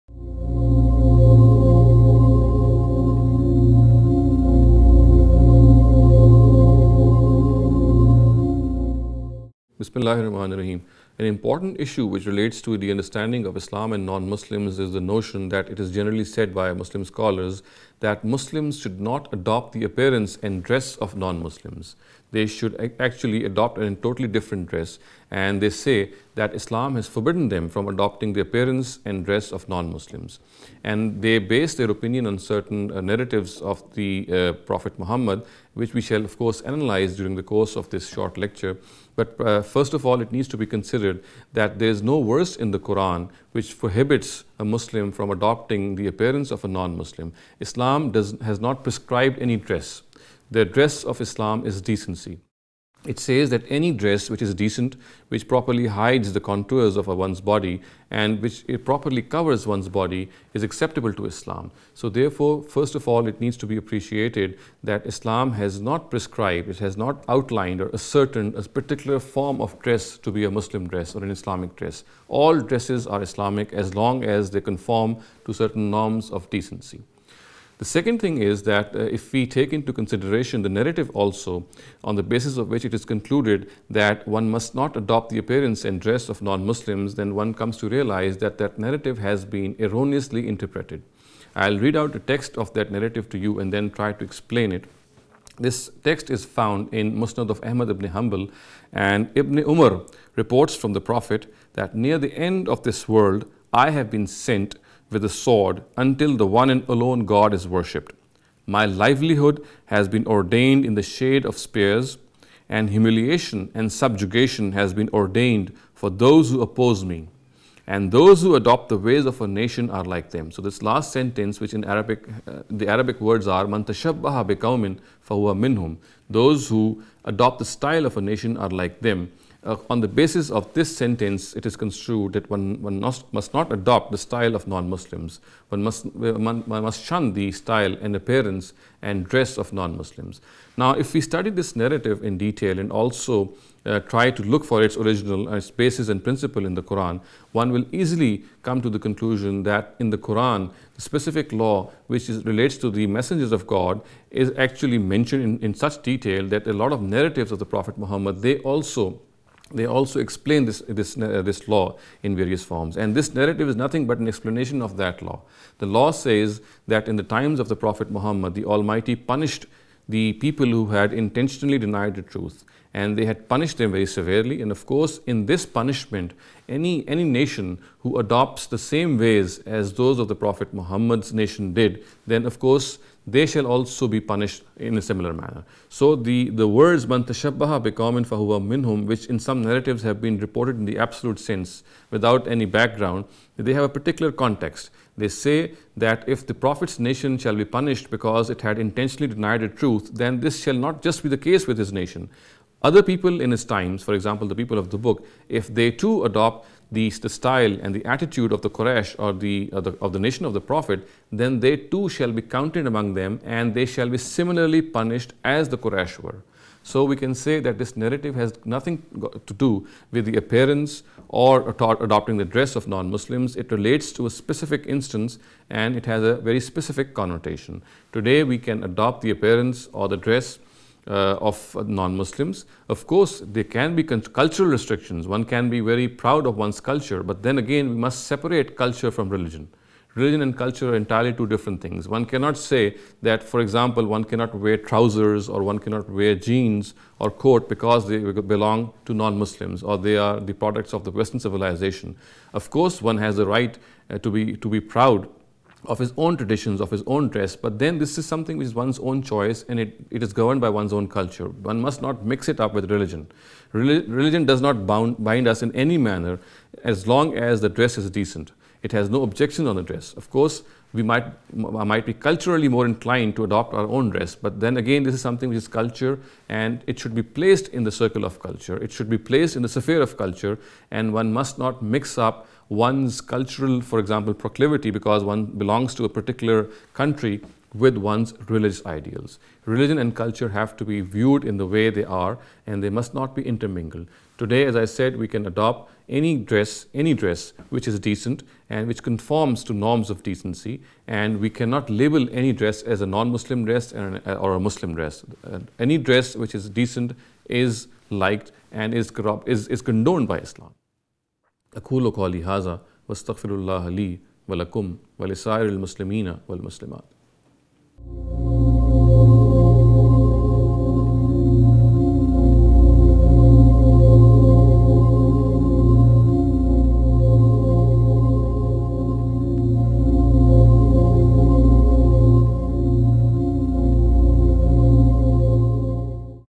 Wearing the Dress of Non Muslims (Some Misconceptions) - Dr Shehzad Saleem
Islam and Non-Muslim Some Misconceptions - a lecture series by Dr. Shehzad Saleem